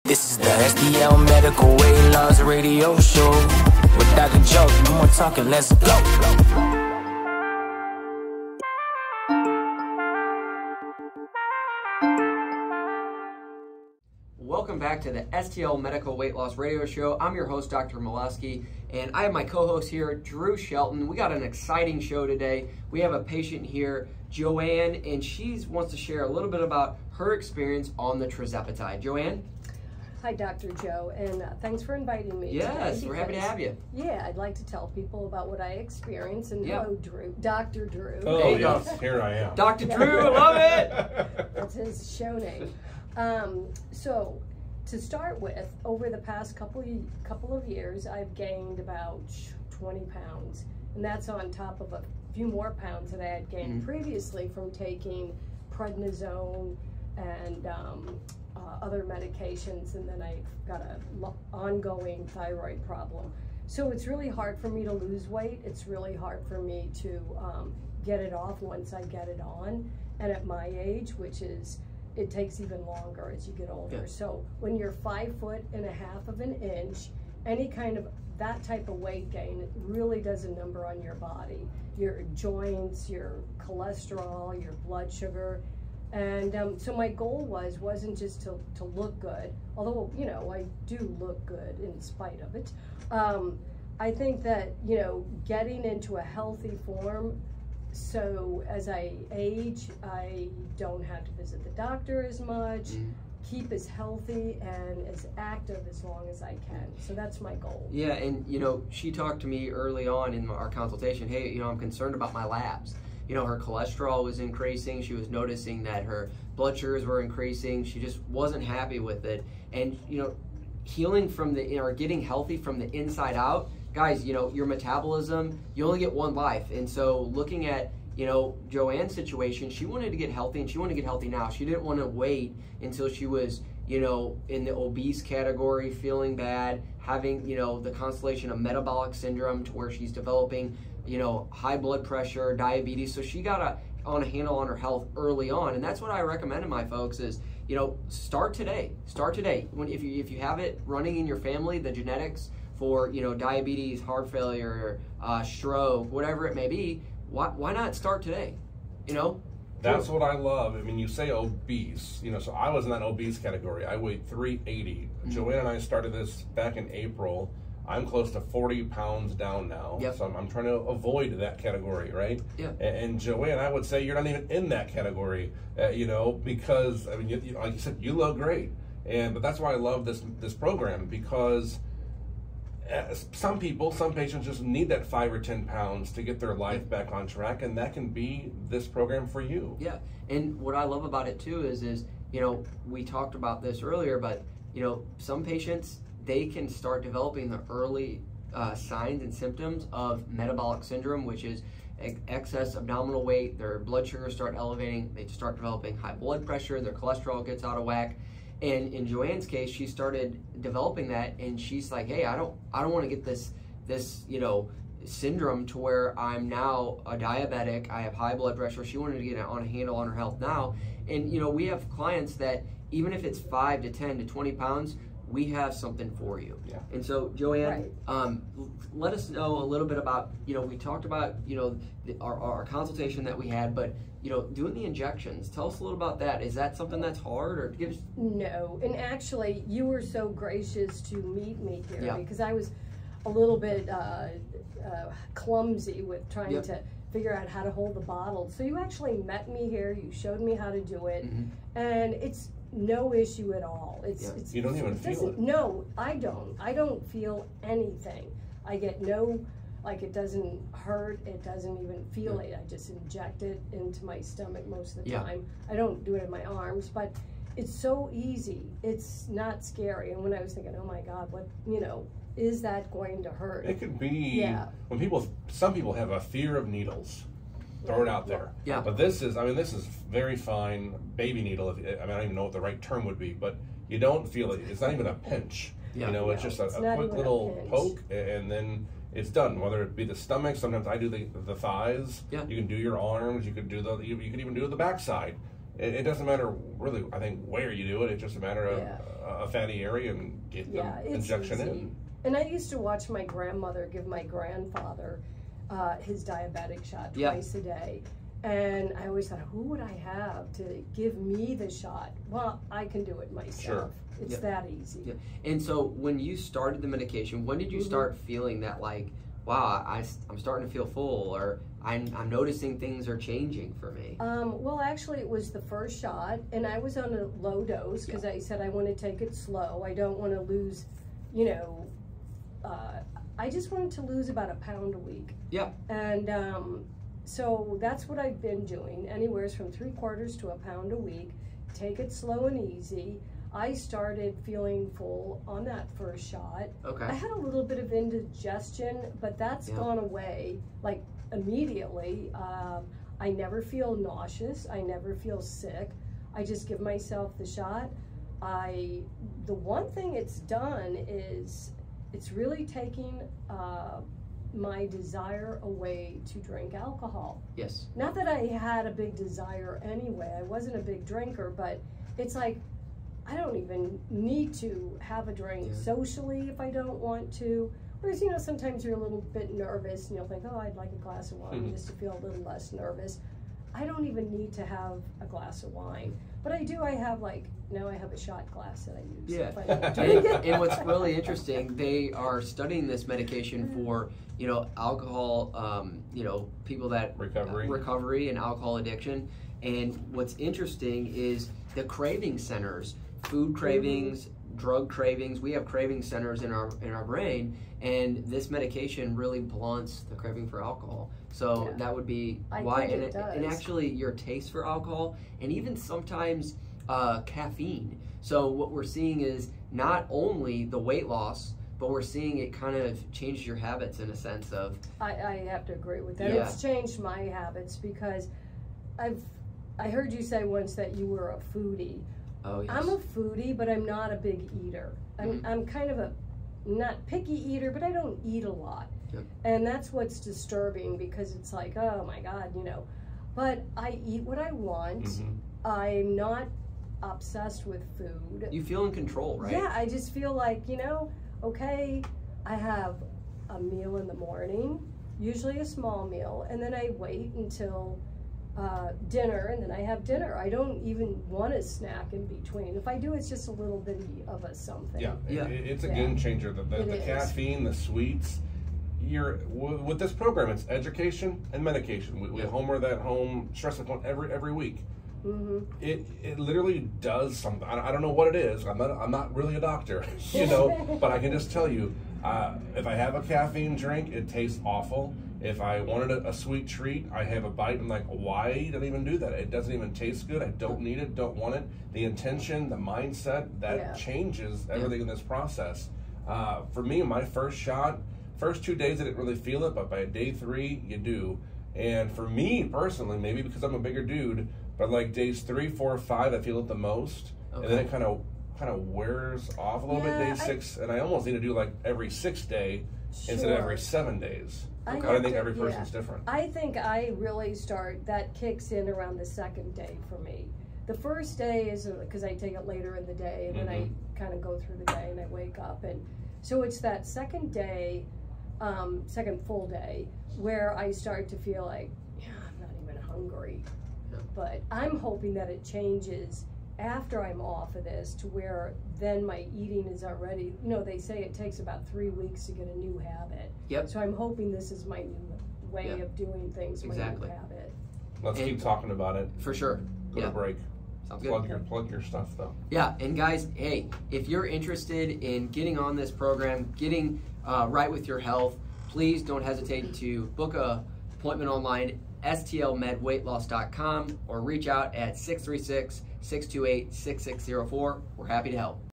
Overcoming weight gain, medications, and thyroid problems with medical weight loss
In this podcast episode, Dr. Moleski and Drew Shelton introduce Joanne, a patient who shares her experience with medical weight loss. Joanne talks about her struggle with weight gain and her ongoing thyroid problem, which makes it difficult for her to lose weight. She emphasizes that her goal is not just to look good, but to improve her overall health and stay active as she ages. Dr. Joe discusses Joanne's concerns about her cholesterol and blood sugar levels and the importance of starting the journey to health early. Drew shares his own weight loss journey, and Joanne discusses her experience with painless injections.

[00:00:44] The patient's weight loss journey - Joanne shares her experience of gaining weight and struggling to lose it due to medication and thyroid problems.
[00:02:01] Importance of starting early for health - Dr. Joe emphasizes the importance of starting to prioritize health early on to prevent metabolic syndrome and other health issues.
[00:03:55] Ease of injections and changes in habits - Joanne discusses how easy it is to administer the injections and how the medication has changed her habits and cravings.